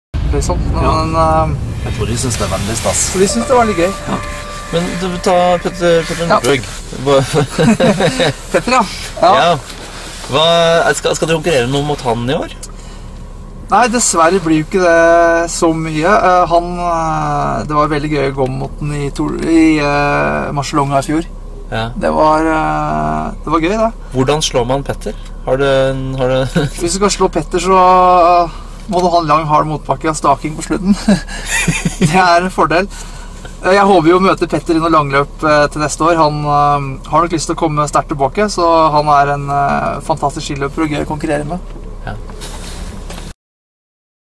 Hvordan slår man Petter Northug?
Aukland betror seg under en reklamefilminnspilling for Mercedes Benz